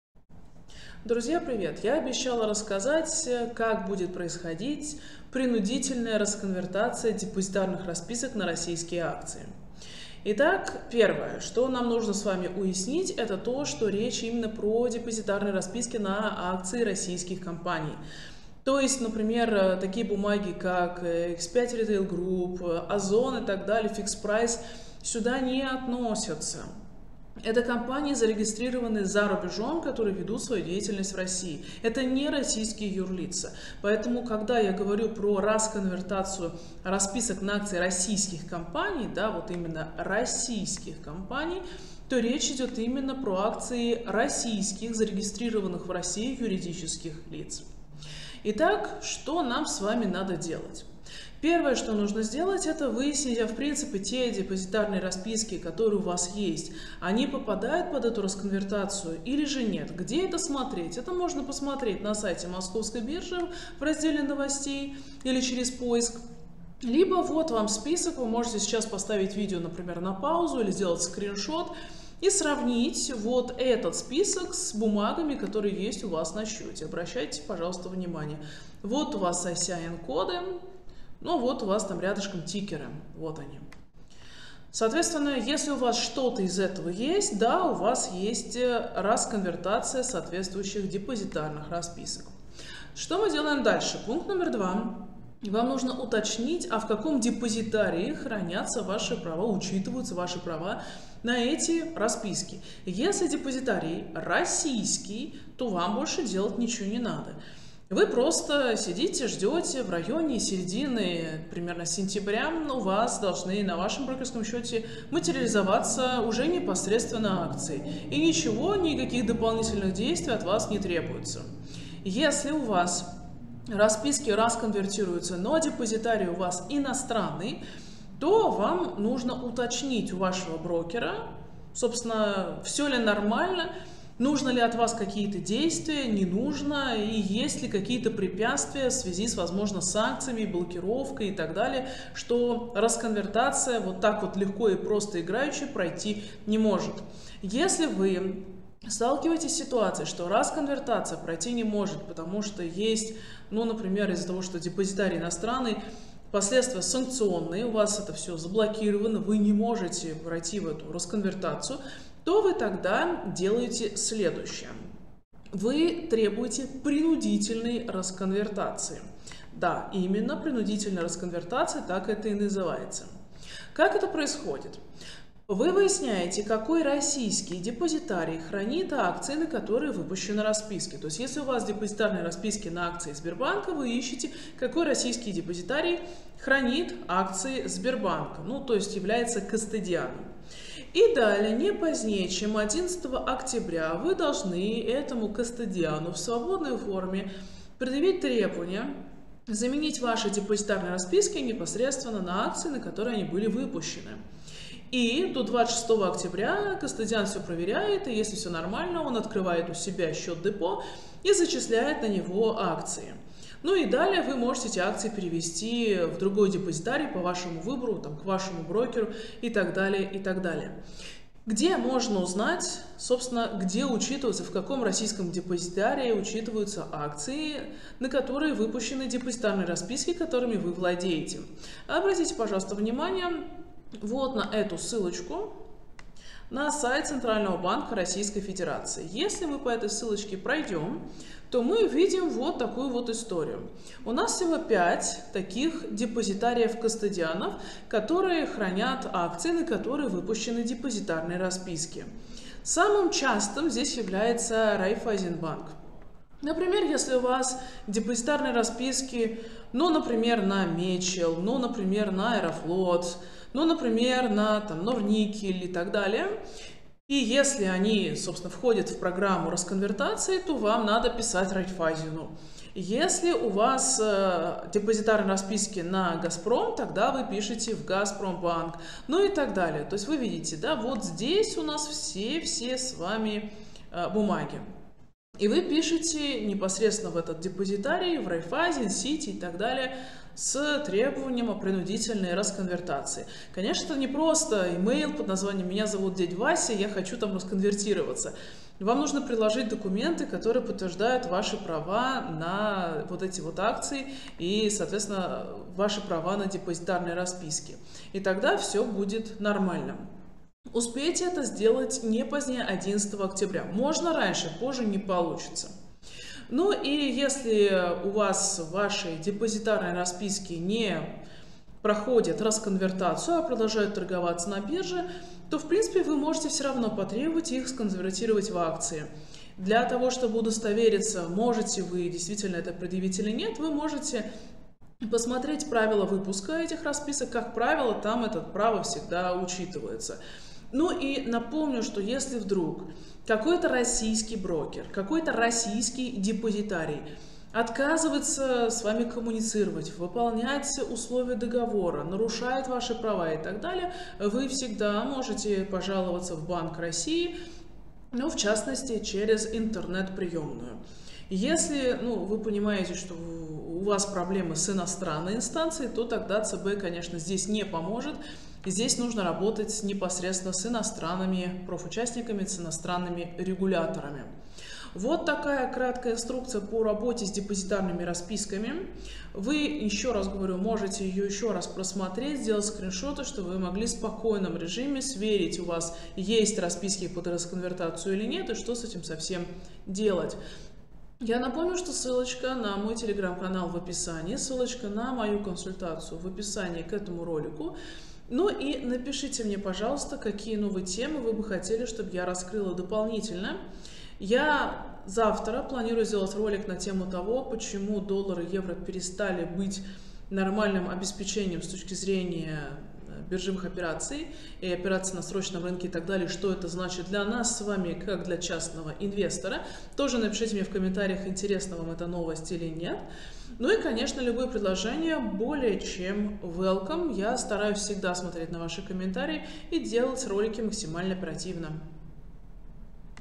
Расконвертация депозитраных расписок: что делать? // Наталья Смирнова
Еженедельные закрытые вебинары с обзором финансовой ситуации в мире и в России, ответы на ваши вопросы